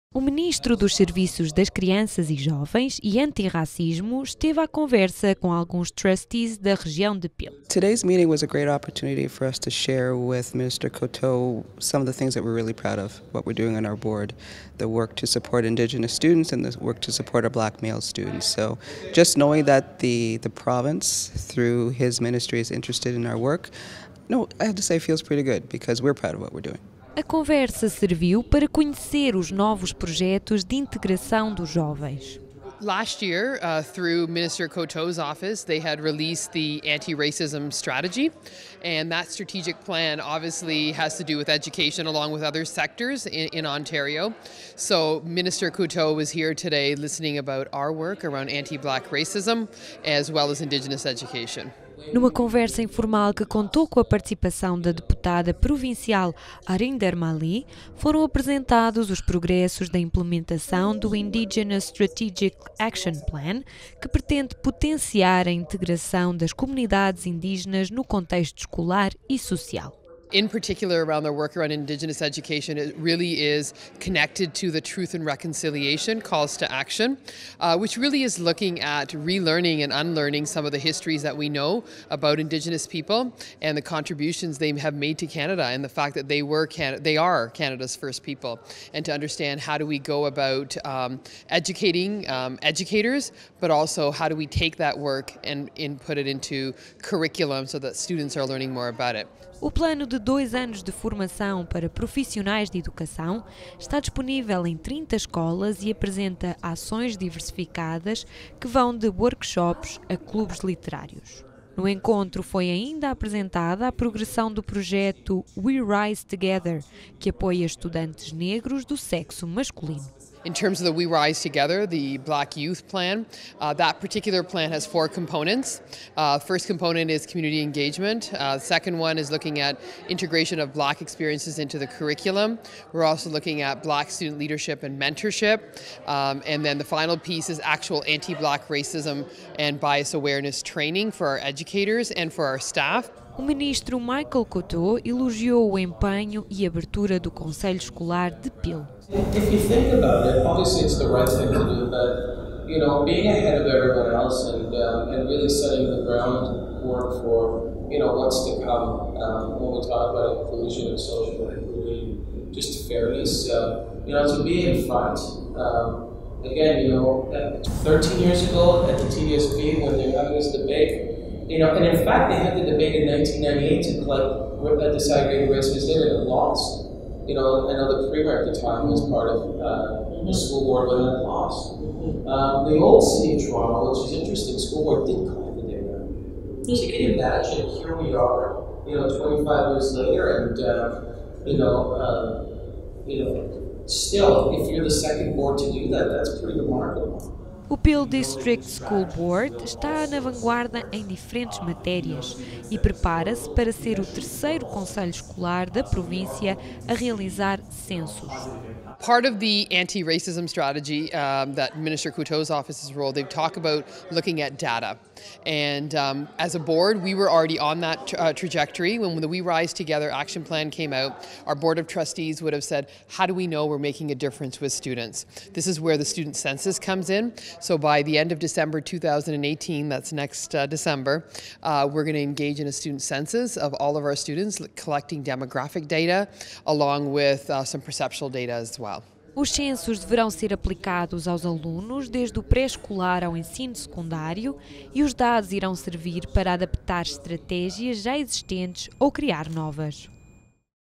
Michael Coteau visits Peel Region
Ontario’s Minister of Children and Youth Services as well as Anti-Racism, Michael Coteau, paid a visit to the cities of Brampton and Mississauga on Nov. 27. The provincial politician met with members of the Peel District School Board to hear about upcoming projects promoting integration and anti-racism among students.

This story was aired on CMCTV on Nov. 28, 2017.

I was responsible for filming and editing the field report.